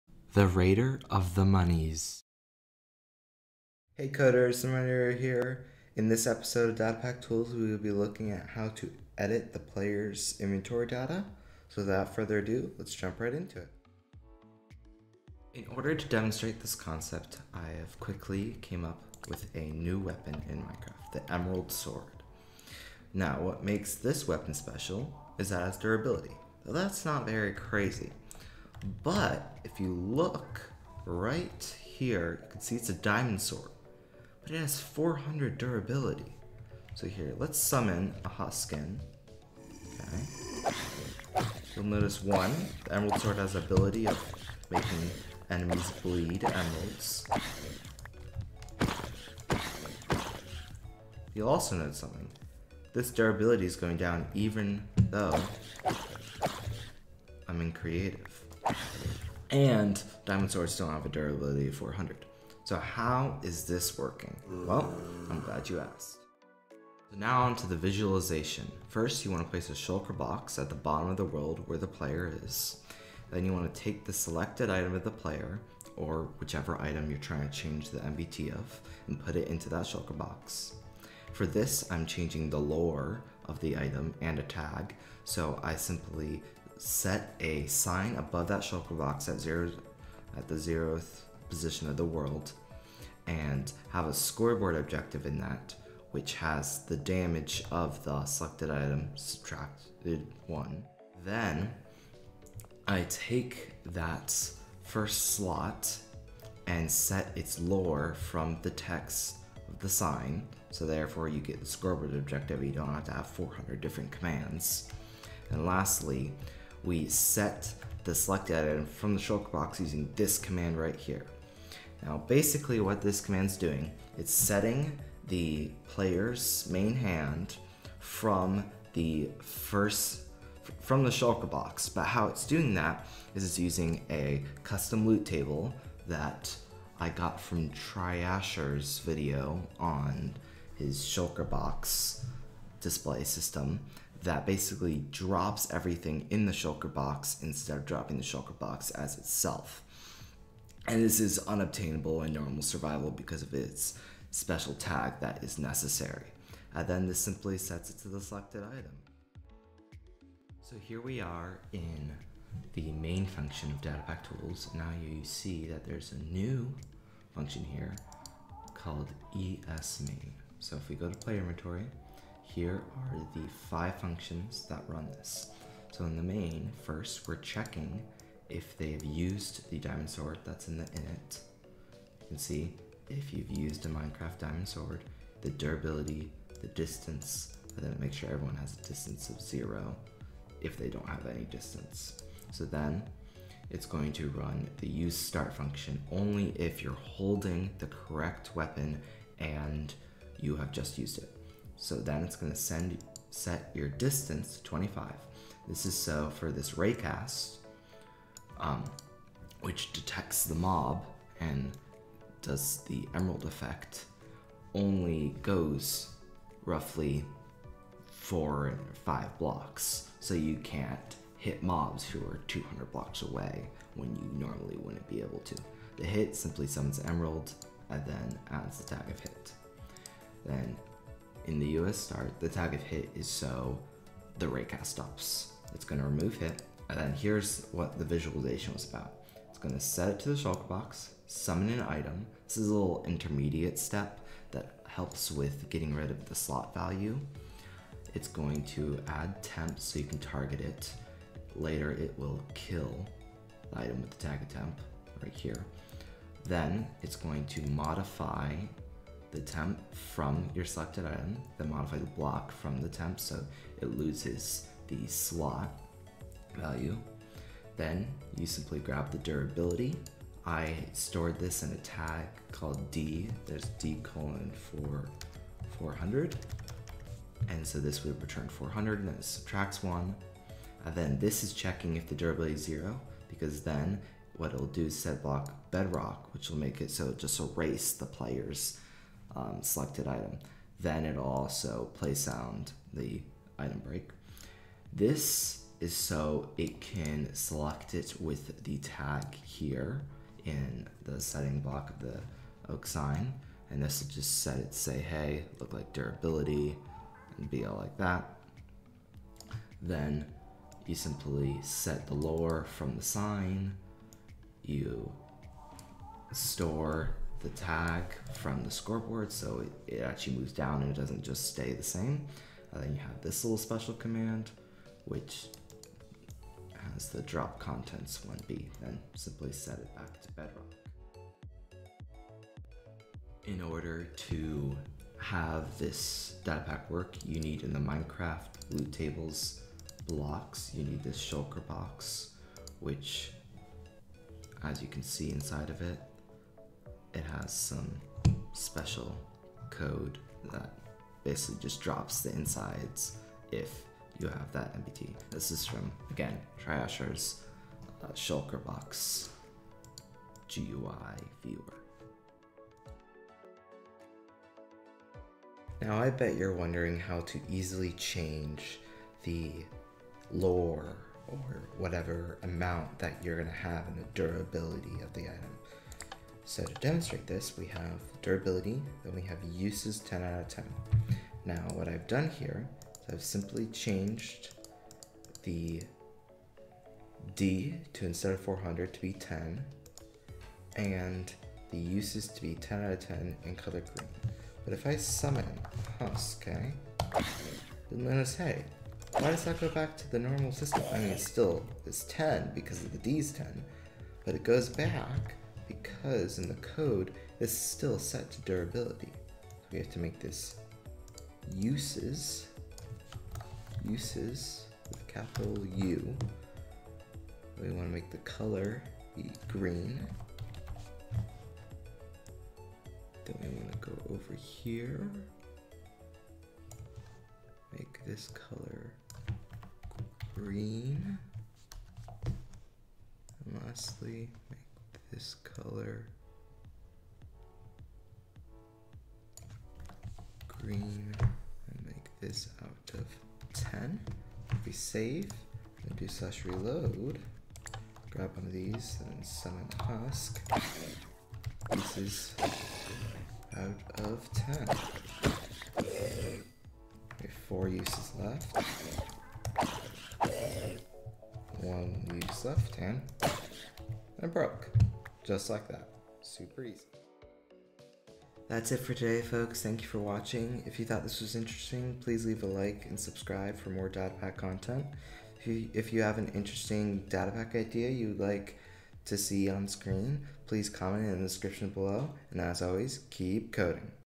Datapack Tools Episode 4: Player Inventory Data
in this Episode of Datapack Tools I explain how to edit player inventory data, a concept I used in the Backpack Datapack! It would really mean a lot!

Put your datapack Ideas in the comment section below!

Intro: 0:00
Demonstration: 0:14
Visualization: 1:06
Coding: 2:40
Loot Table: 5:31
How to Change: 6:04
Outro: 7:47